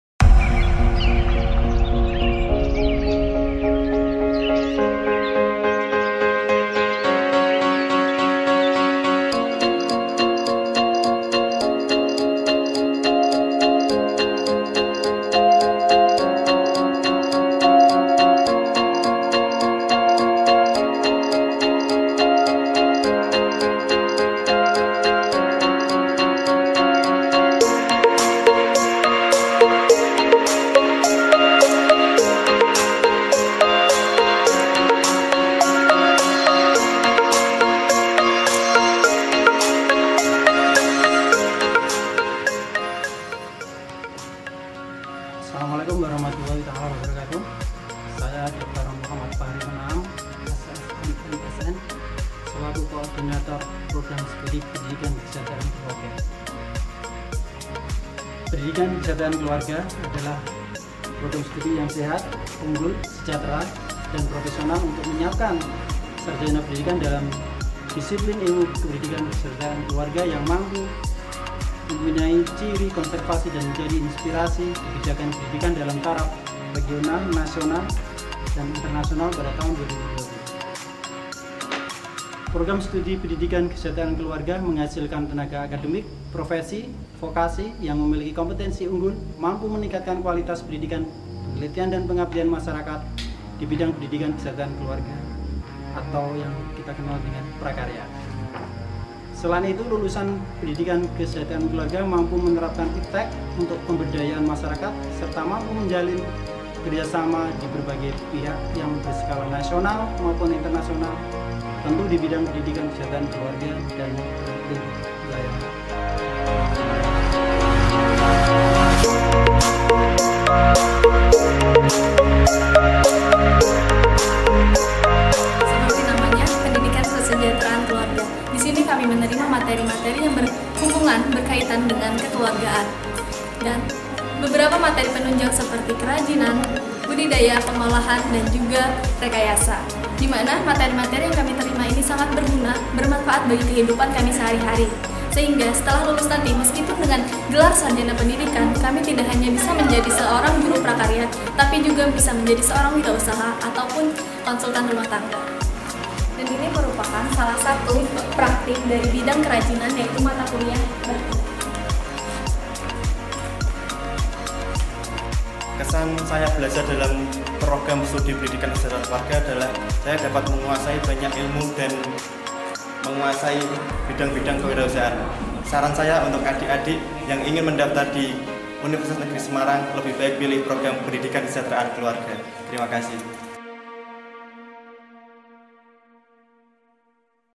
Profil Prodi Pendidikan Kesejahteraan Keluarga UNNES 2020
Profil Program Studi S1 Pendidikan Kesejahteraan Keluarga Universitas Negeri Semarang 2020.

Prodi PKK merupakan keluarga dari jurusan PKK yang menghasilkan lulusan lulusan kreatif.